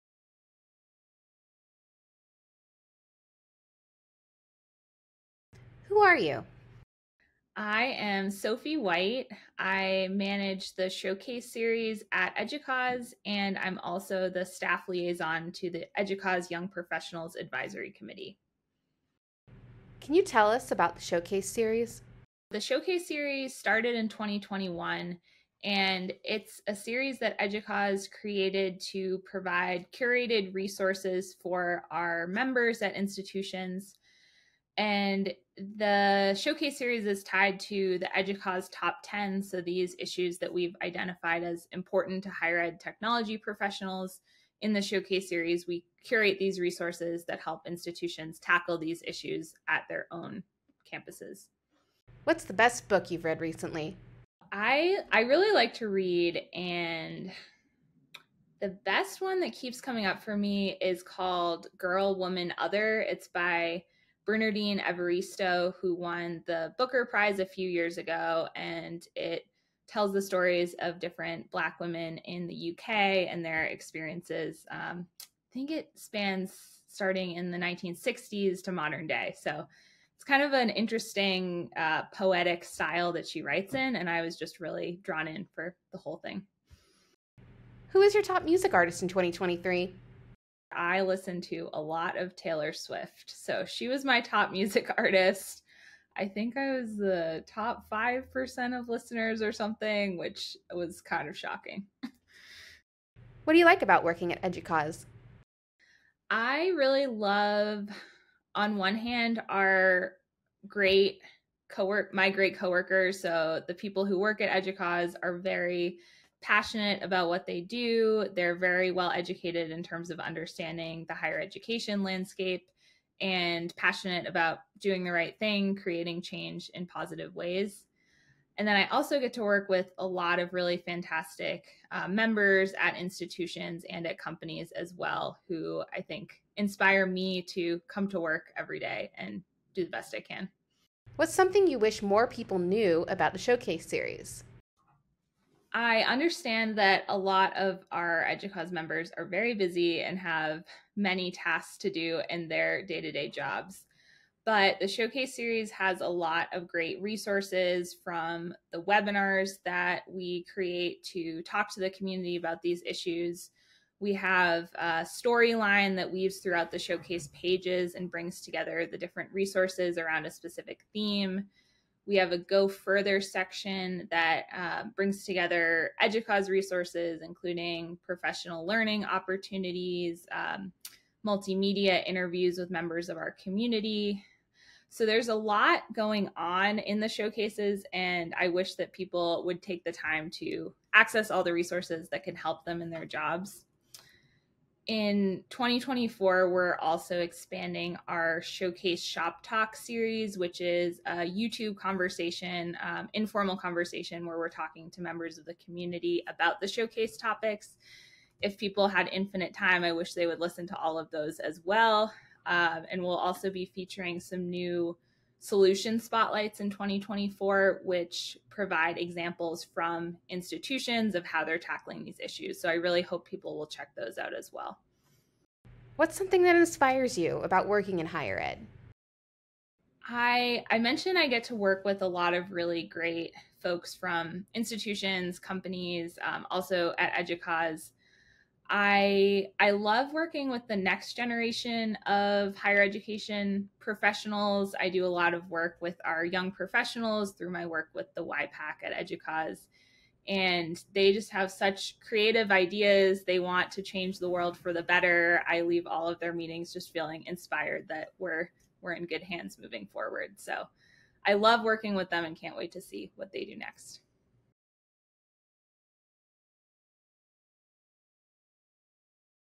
7 Questions With... EDUCAUSE Showcase Program Manager Sophie White
What's coming up for the Showcase in 2024? What inspired Sophie about working in higher education? And who was her top musical artist of 2023?